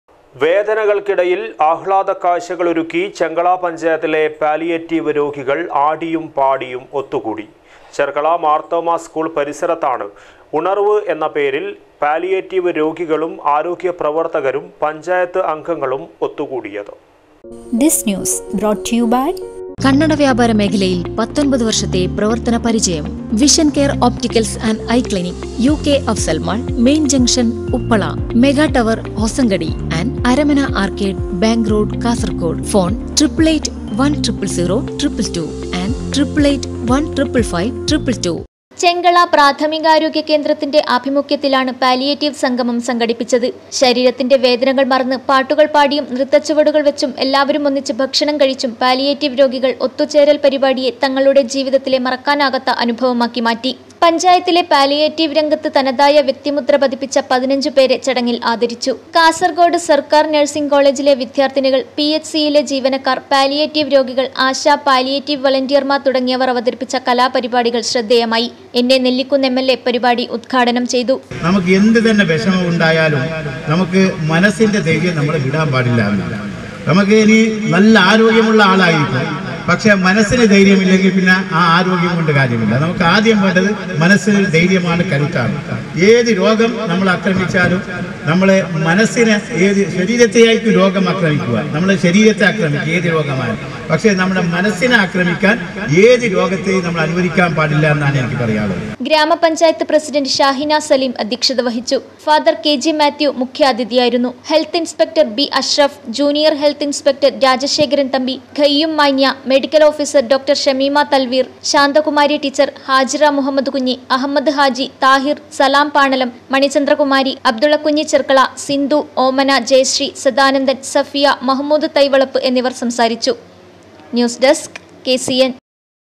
പാലിയേറ്റീവ് രോഗികള്‍ ആടിയും പാടിയും ഒത്തുകൂടി...
വേദനകള്‍ക്കിടയല്‍ ആഹ്ലാദക്കാഴ്ച്ചകളൊരുക്കി ചെങ്കള പഞ്ചായത്തിലെ പാലിയേറ്റീവ് രോഗികള്‍
ആടിയും പാടിയും ഒത്തുകൂടി. ചെര്‍ക്കള മാര്‍ത്തോമ സ്‌കൂള്‍ പരിസരത്താണ് 'ഉണര്‍വ്വ്' എന്ന പേരില്‍
പാലിയേറ്റീവ് രോഗികളും ആരോഗ്യപ്രവര്‍ത്തകരും പഞ്ചായത്ത് അംഗങ്ങളും ഒത്തുകൂടിയത്.